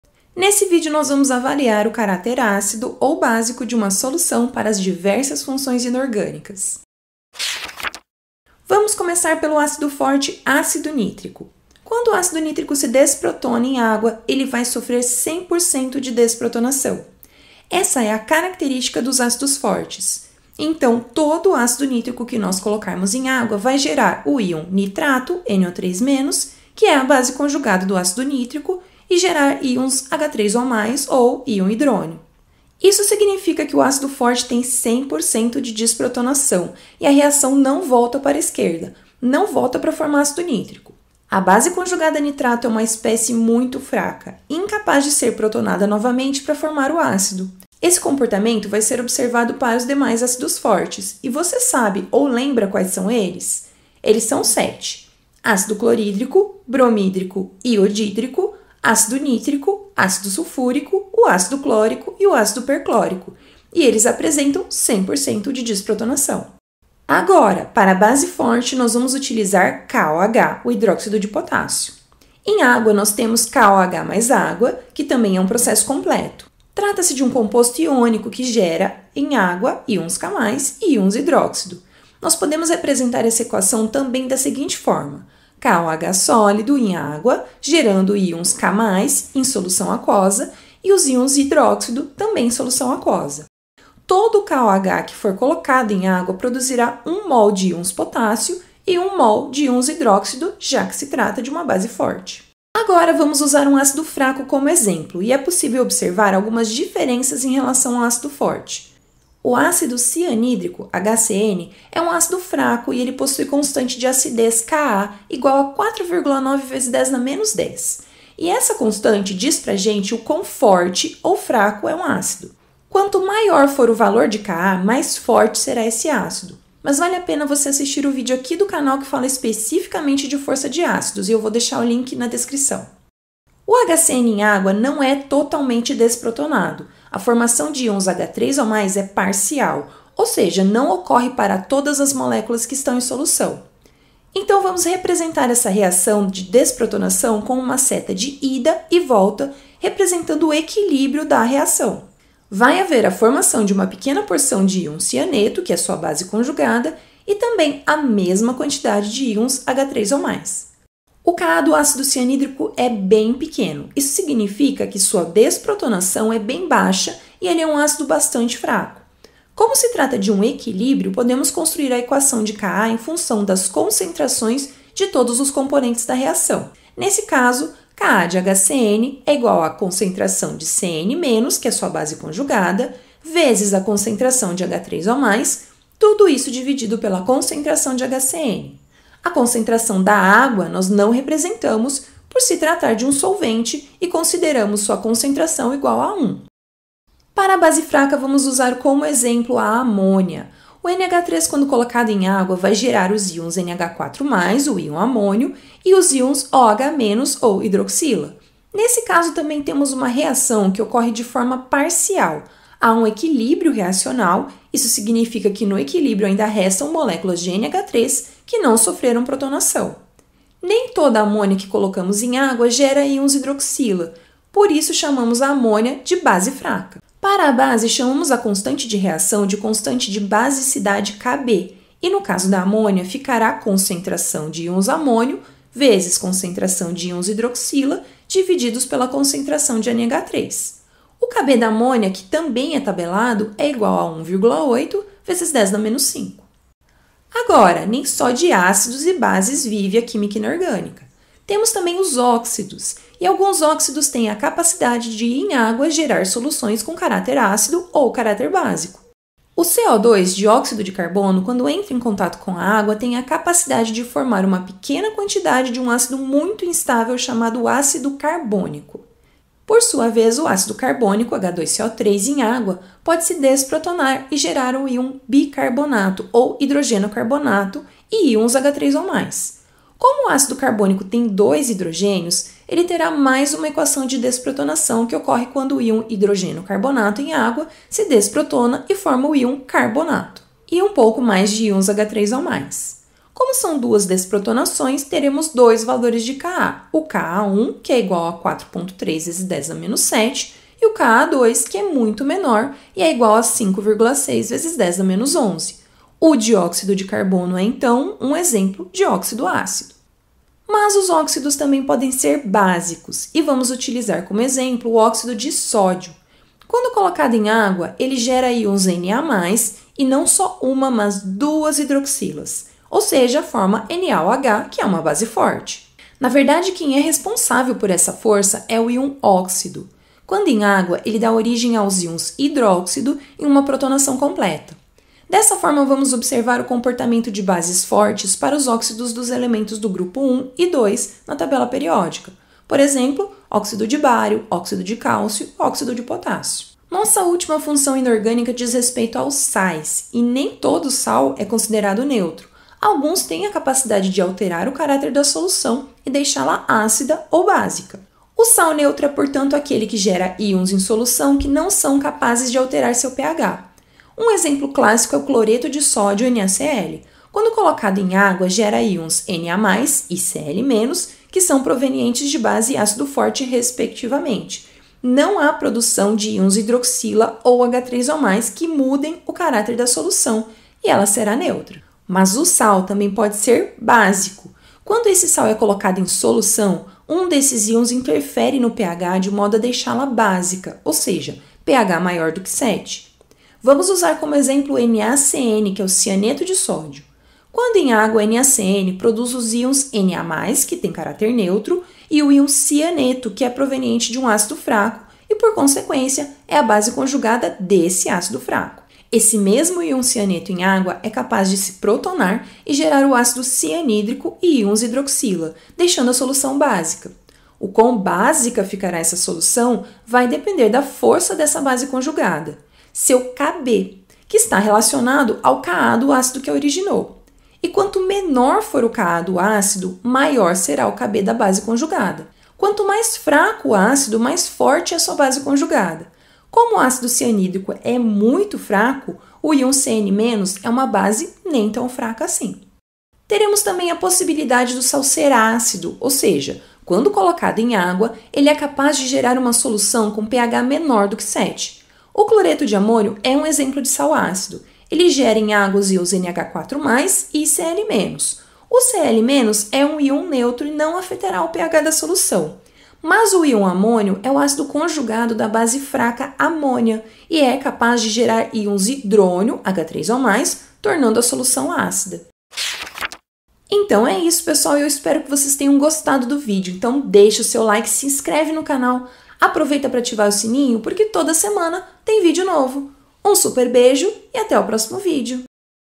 Ácido ou básico? O caráter das funções inorgânicas
Para entender o caráter ácido ou básico (ou até neutro) de uma solução contendo compostos inorgânicos é preciso entender o comportamento destas espécies em água.
Os ácidos fortes têm como característica principal, serem totalmente desprotonados em água. Já os ácidos fracos têm apenas desprotonação parcial, ela ocorre no equilíbrio e apresenta constante de acidez. Já as bases fortes apresentam protonação total e as bases fracas são apenas parcialmente protonadas. A protonação das bases fracas também é representada no equilíbrio e apresentam constante de basicidade.
Óxidos também podem apresentar característica ácida quando geram um ácido em solução e básica quando geram base (íons hidróxido) em solução. Já os sais precisam ser analisados. Isso porque cada cátion e ânion gerado em solução, pode ou não influenciar em seu caráter. O sal portanto, pode ser ácido, básico ou neutro.